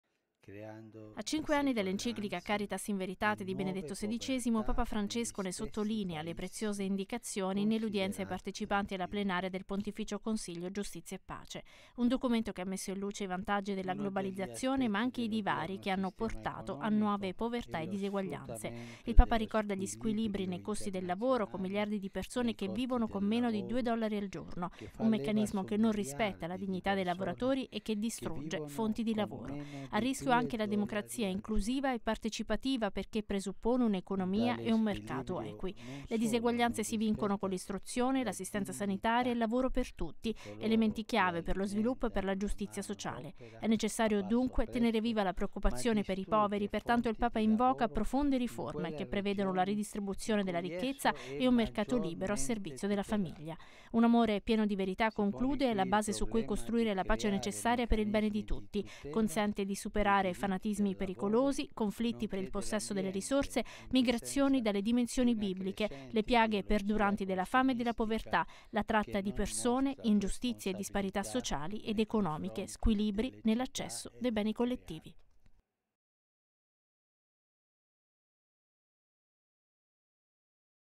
Incontro partecipanti alla Plenaria del Pontificio Consiglio Giustizia e Pace 02-10-2014
Profonde riforme contro le diseguaglianze sociali

Papa Francesco ha ricevuto stamani i partecipanti alla Plenaria del Pontificio Consiglio Giustizia e Pace. Lo sviluppo dei popoli nasce dall’istruzione, dal lavoro e dal welfare